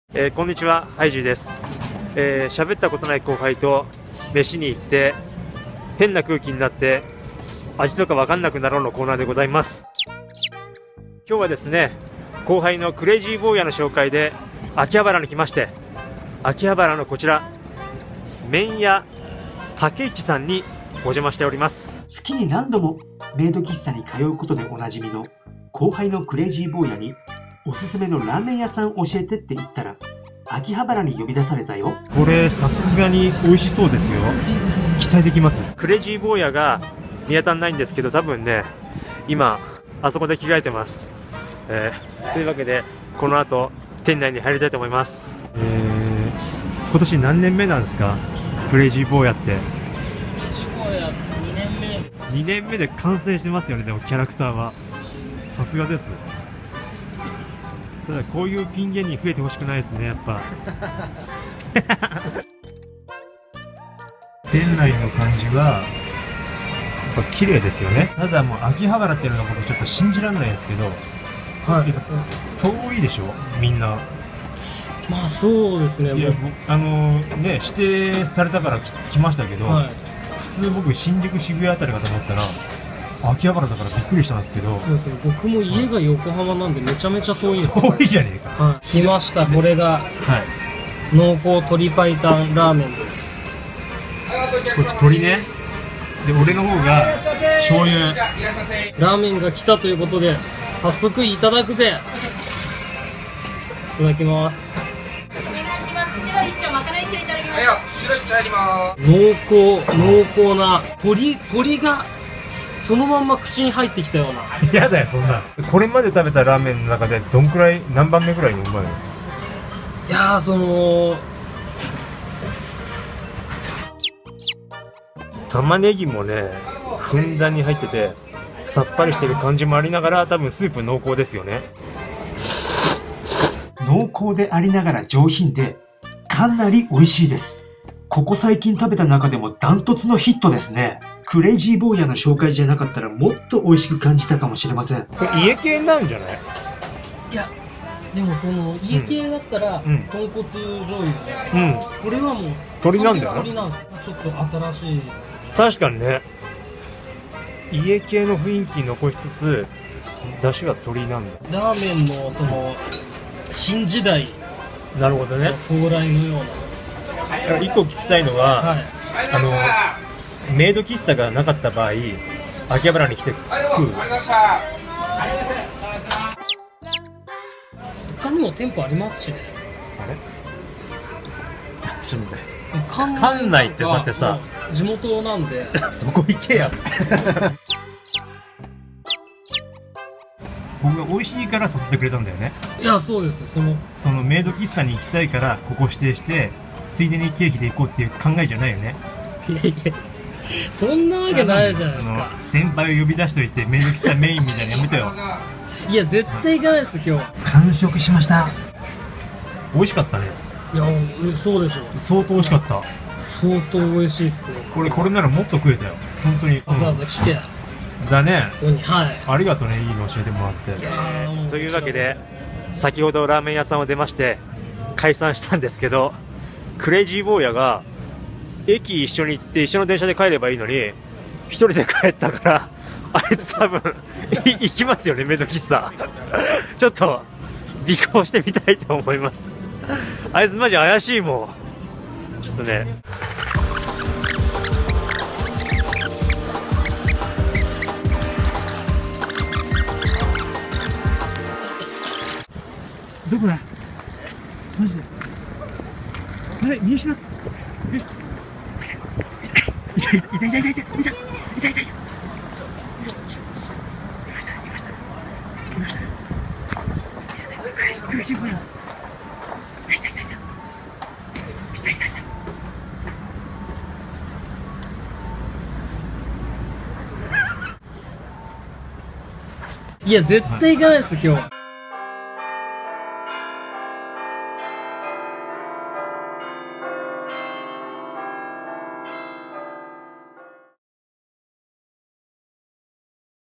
通いたくなるラーメン店【麺屋 武一】の激ウマ濃厚鶏だしスープ！
今回は、ピン芸人の後輩にオススメのラーメン屋さんを紹介してもらってきました！
まさかの激ウマっぷりに、驚きを隠せませんでした！

☆お店情報
麺屋 武一 秋葉原店
東京都千代田区外神田3-13-5
[月～土]11:30～23:00(月曜祝日の時は22:00まで)
[日]11:30～22:00(月曜祝日の時は23:00まで)

毎週水曜日には自作アニメをアップロードしています。